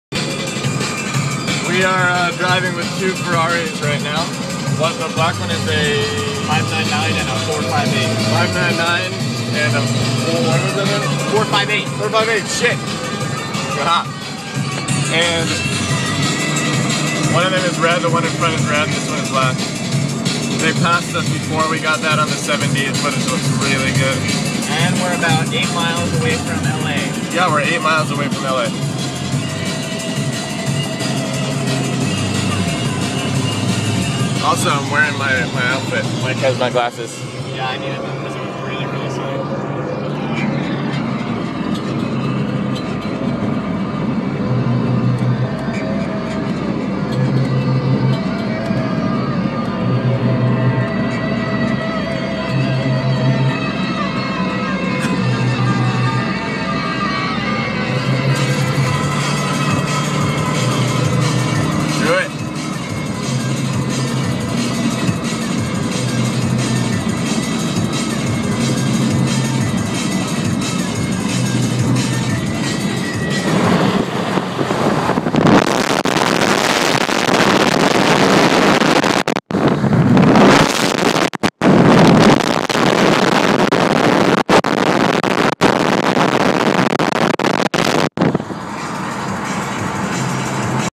GUMBALL 2012 UPDATE: Day Seven Driving with two Ferraris
Shot with an iPhone for immediate upload so please forgive the quality. Okay? Great. Thanks.